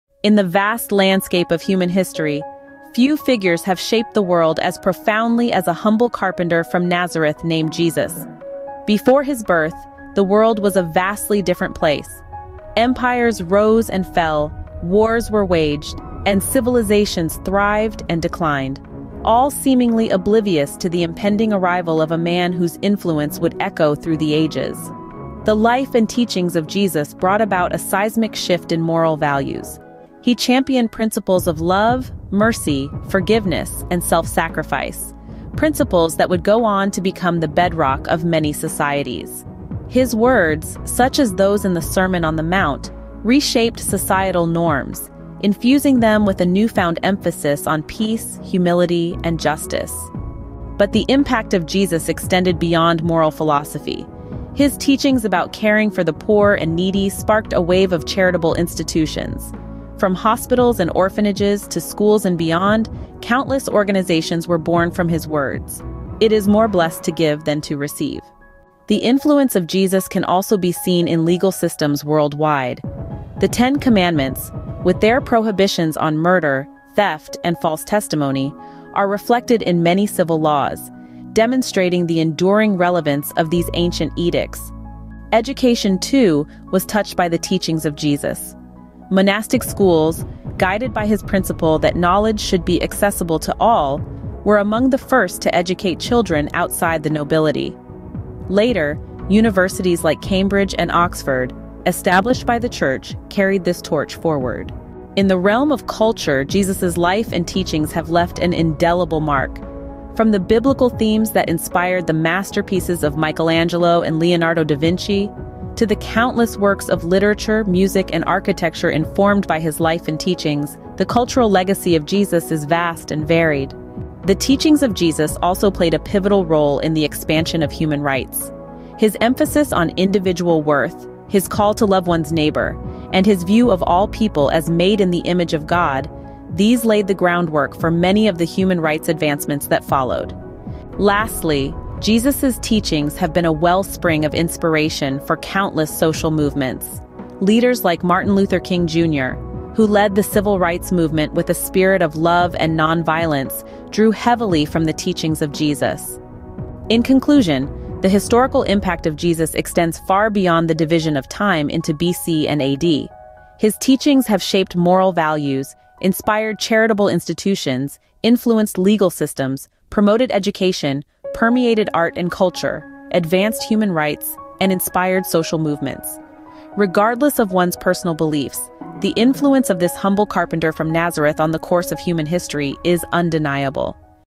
Beyond Belief: How Jesus Shaped History and Impacted Atheists Across Time
In this thought-provoking video, join us as we delve deep into the profound historical impact of Jesus, going beyond the division of time. Discover fascinating historical interpretations and explore religious texts that shed light on the influence of Jesus throughout history. From his teachings to his miracles, this exploration will leave you with a new perspective on this iconic figure of faith. Don't miss out on this captivating journey through time! Get ready to unravel the extraordinary legacy of Jesus like never before.

THIS IS WHERE REAL LIFE INTERACTS WITH THE VIRTUAL WORLD

CLAIM YOUR PLACE IN THE WORLD NOW

Go to https//imreal.life to join.